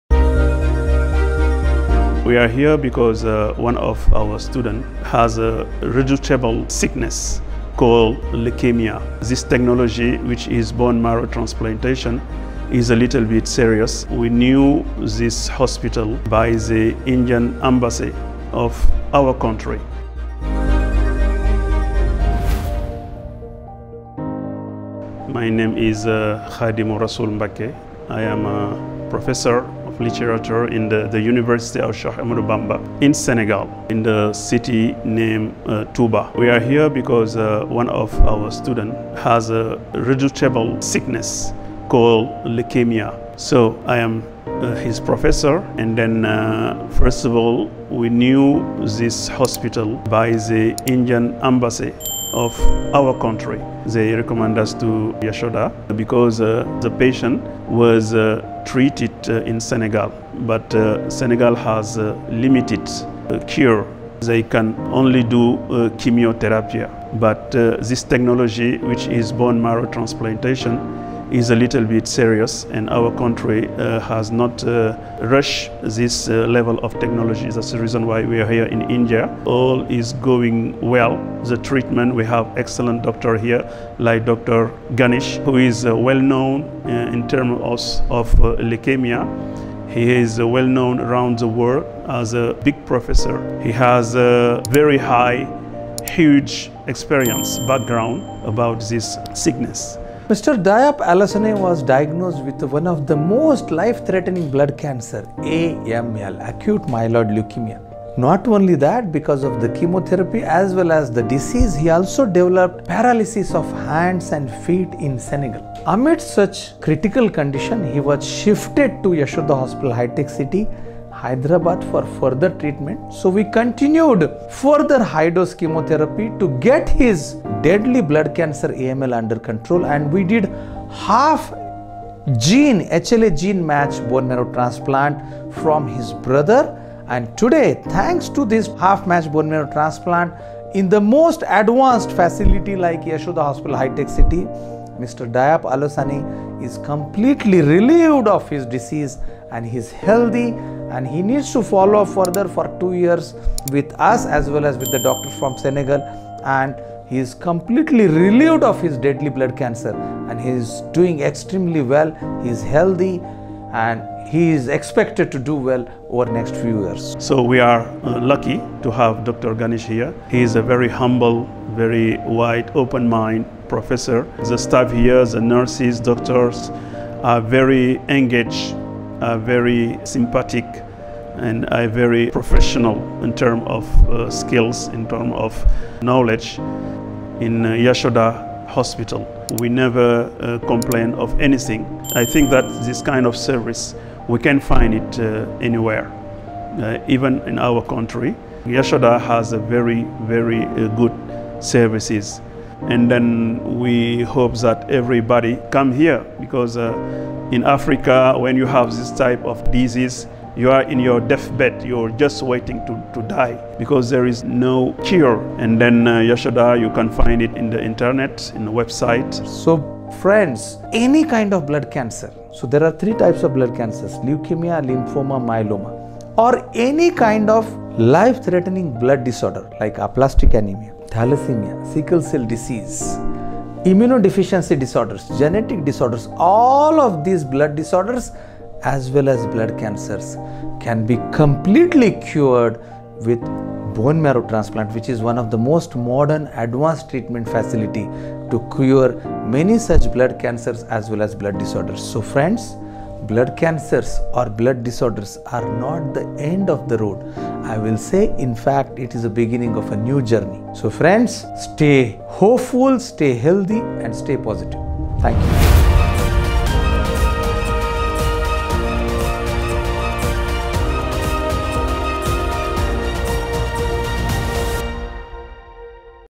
Treatment for Acute Myeloid Leukemia (AML) | Half-Match Bone Marrow Transplant
Acute Myeloid Leukemia (AML) is a type of cancer that starts in the blood and bone marrow, characterized by the rapid growth of abnormal white blood cells. These cells crowd out normal blood cells, leading to symptoms such as fatigue, frequent infections, and easy bruising or bleeding. The exact cause of AML is often unknown, but factors like genetic mutations, previous chemotherapy, and exposure to certain chemicals can increase the risk. Diagnosis typically involves blood tests, bone marrow biopsies, and genetic testing to identify the specific type of AML. Treatment usually begins with intensive chemotherapy to destroy the cancer cells, followed by additional therapies such as targeted treatments, immunotherapy, or stem cell transplants to reduce the risk of relapse.

A Half-Match Bone Marrow Transplant, also known as a haploidentical transplant, is a treatment option for AML when a fully matched donor is not available. In this procedure, the patient receives healthy stem cells from a donor who is only a partial match, such as a parent, sibling, or child. Before the transplant, the patient undergoes chemotherapy or radiation to destroy the existing bone marrow. The healthy donor stem cells are then infused into the patient’s bloodstream, where they migrate to the bone marrow and begin producing new, healthy blood cells. This approach increases the chances of finding a suitable donor, making it a valuable option for AML patients who need a transplant. Recovery involves close monitoring to manage complications such as graft-versus-host disease (GVHD), where the donor cells attack the patient’s tissues. Despite the challenges, a half-match transplant offers hope for long-term remission and survival in AML patients.

Mr. Diop Alassane from Senegal successfully underwent Half-Match Bone Marrow Transplant for Acute Myeloid Leukemia (AML) at Yashoda Hospitals, Hyderabad, under the supervision of Dr. Ganesh Jaishetwar, Sr. Consultant Hematologist, Hemato-Oncologist & Bone Marrow Transplant Physician.